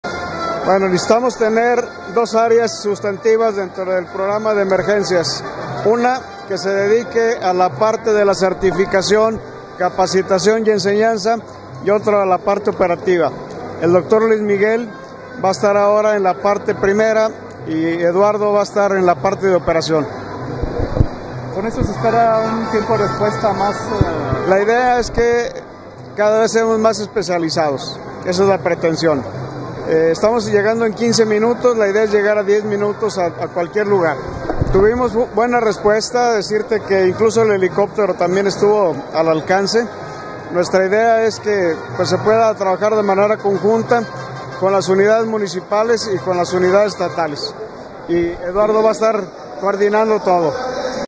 Buscan reducir a 10 minutos tiempo de atención en emergencias
* Actualmente se llega en 15 minutos a cualquier lugar donde se requiera atención de urgencia
* El Doctor Eduardo Álvarez se hace cargo ya del área operativa de emergencias en todo el estado de Aguascalientes

Aguascalientes, Ags. 25 de septiembre 2024.- Con el objeto de reducir a solo 10 minutos la presencia de paramédicos y atención especializada en cualquier emergencia registrada en el territorio estatal, de determinó relanzar el nombramiento del doctor Eduardo Álvarez como titular del área operativa de urgencias, confirmó el Secretario de Salud de la entidad, Rubén Galaviz Tristán.
Explicó la necesidad de fortalecer también la certificación, capacitación y enseñanza, que ha sido fortalecida.
Actualmente, dijo, el tiempo de respuesta es de 15 minutos, sin embargo se espera reducir este lapso a solo 10.